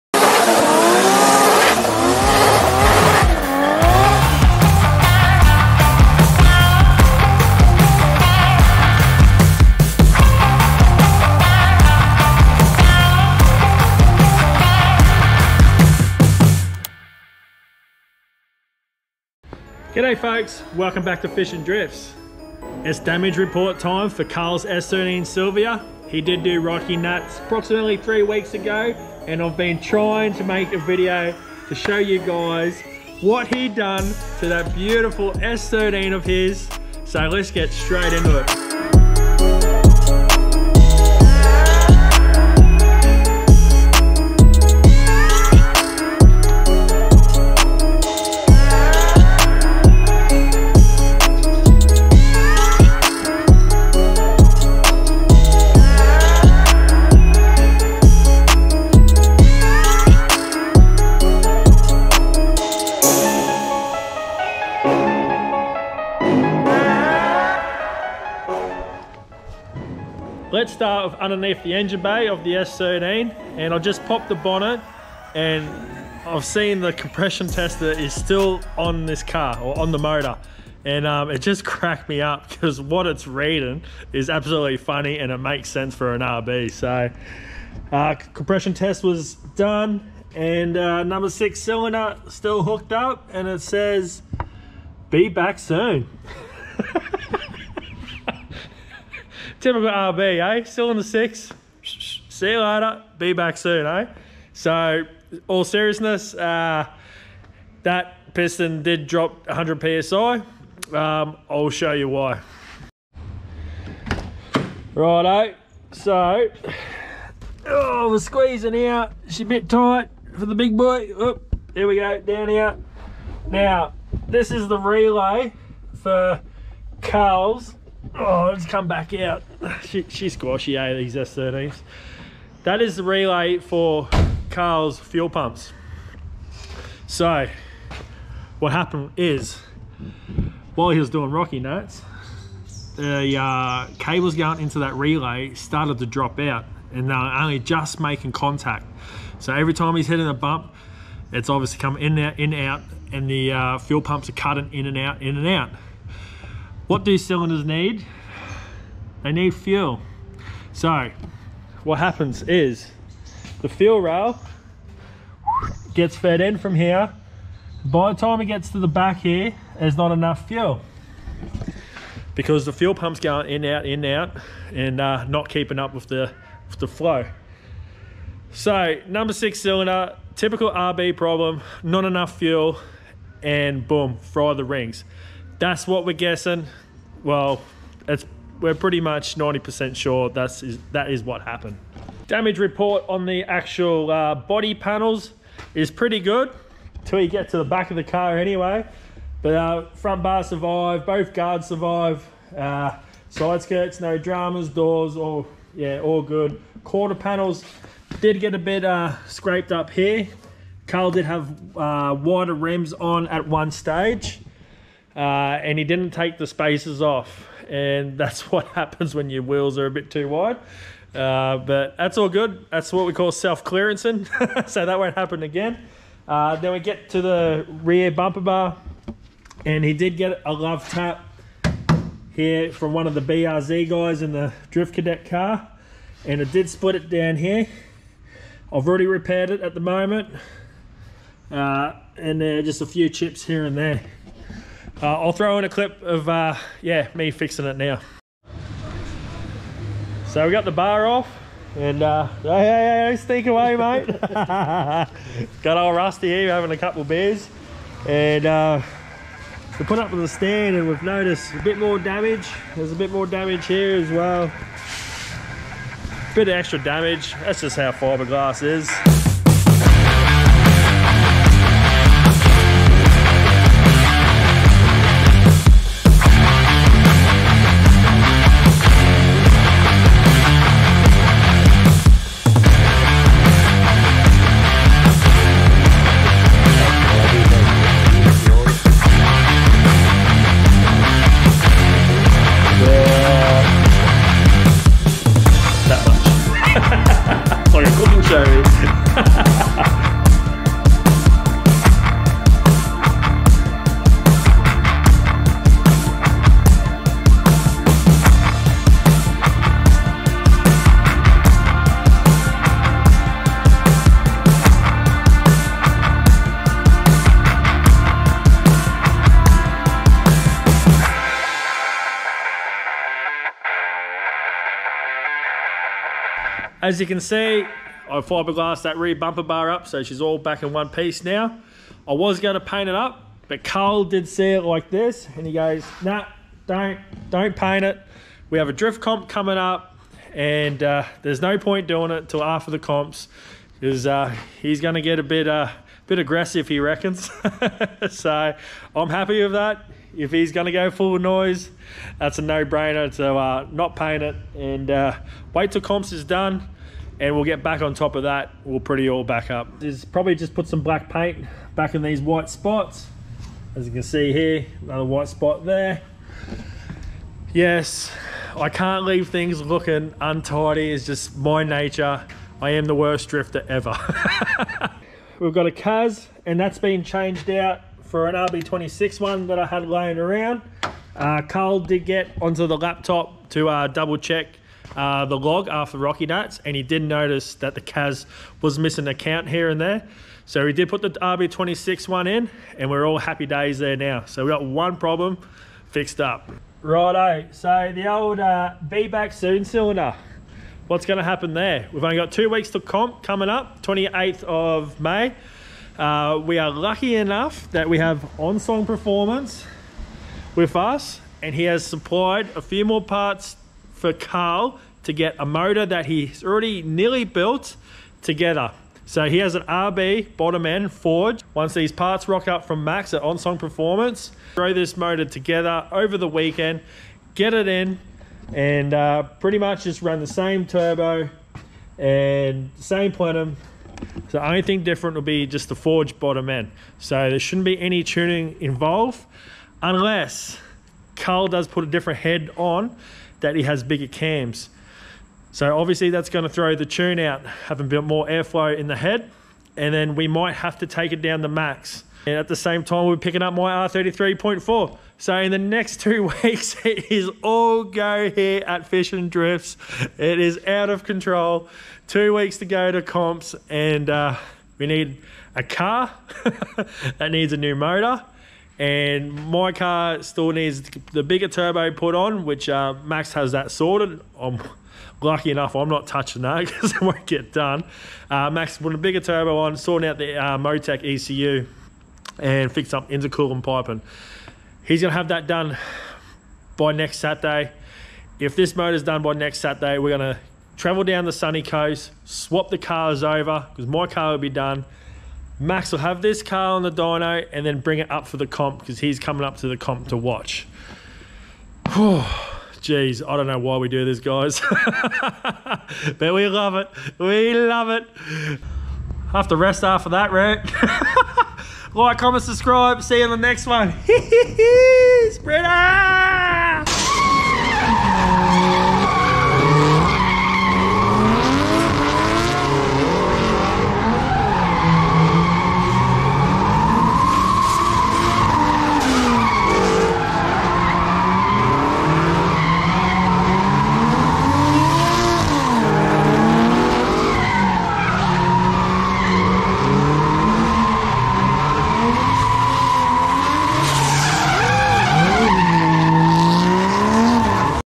NISSAN S13 SILVIA DAMAGE REPORT!!!
KARLS S13 SILVIA IS BACK INTO THE SHOP AFTER 2022 ROCKYNATS, WHAT WENT WRONG ??

FOR ALL YOUR FISH & DRIFT MERCH